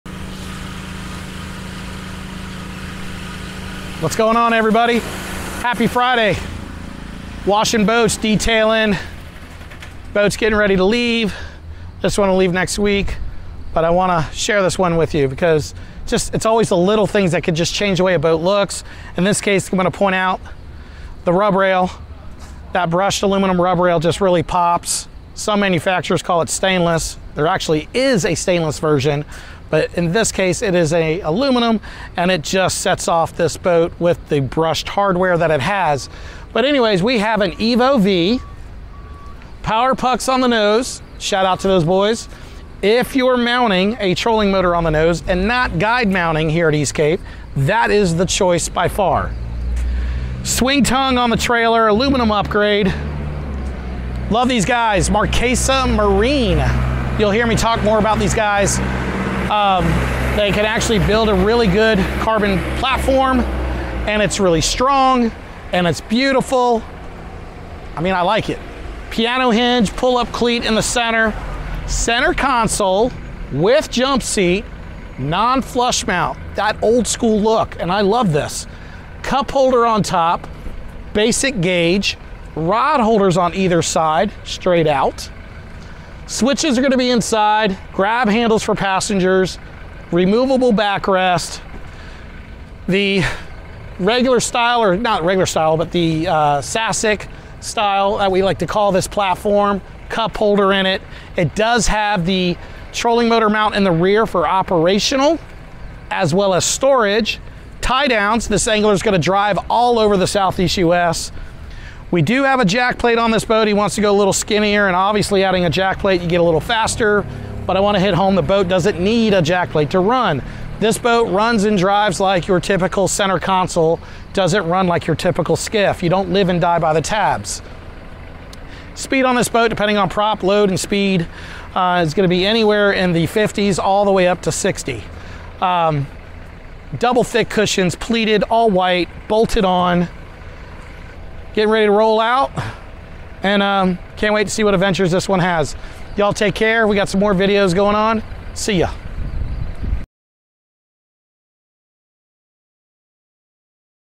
2023 East Cape EVO with 115 Mercury ProXS
Kevin does a full walk around on one of the latest EVO's to leave the shop.  Thank you for letting us build your dream skiff.